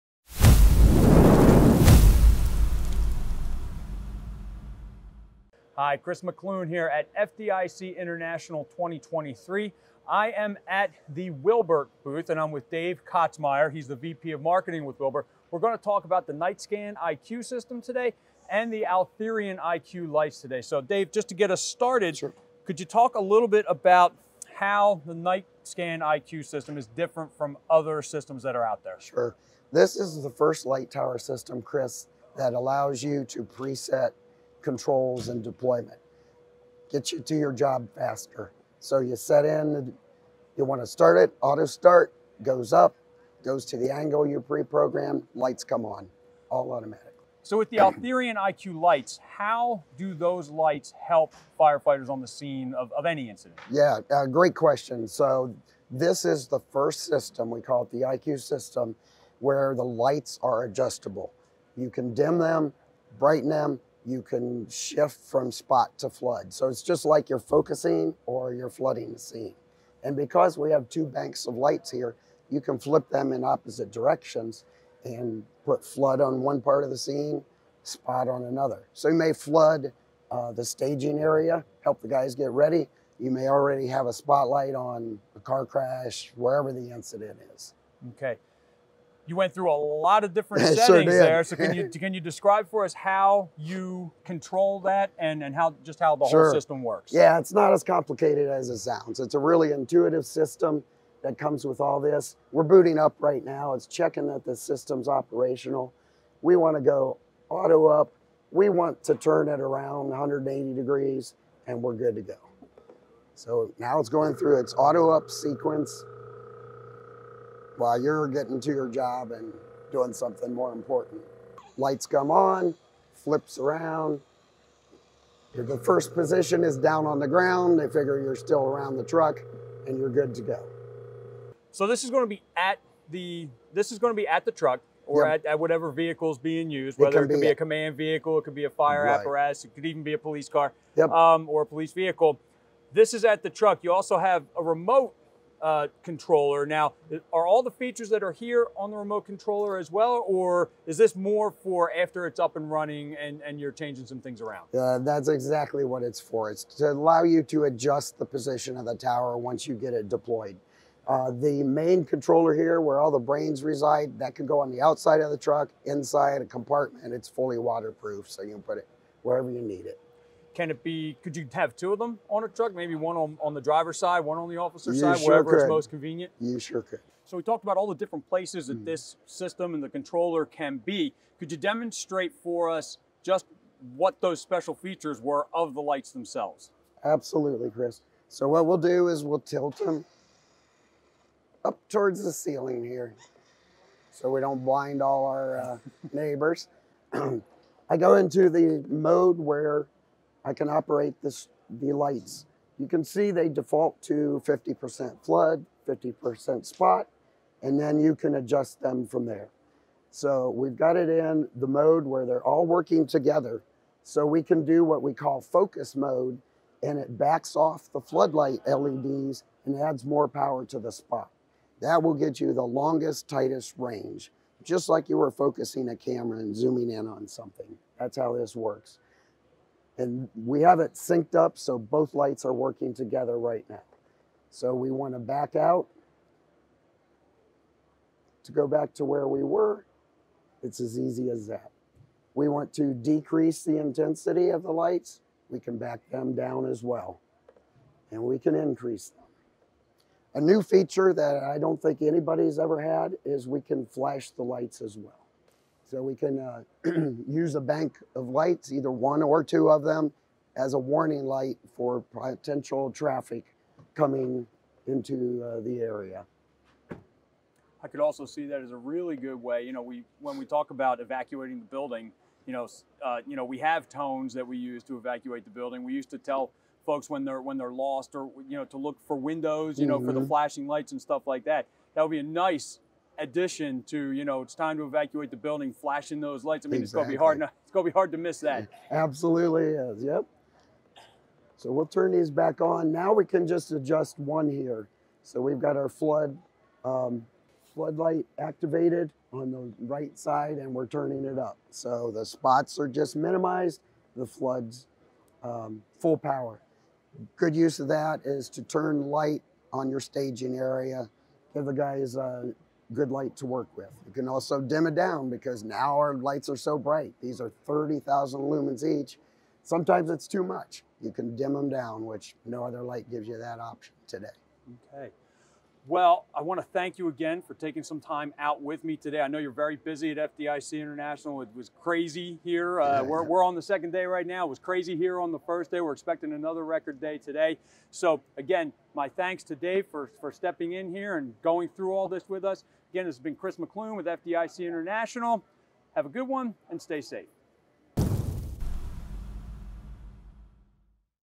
Will-Burt’s Night Scan IQ System – The Intelligent Light Tower
The Night Scan IQ System gives you 100% control of your scene illumination.
Each lamp has a combination of powerful spot and flood LEDs that you control from the LCD screen.
You can control two banks of lights independently. The staging area can be washed with floodlighting by one bank of lights. The second can be directed at the work area where the lights can be dialed to spotlight mode.
Alert oncoming traffic to your presence by selecting flash mode.
Night Scan IQ gives you total control!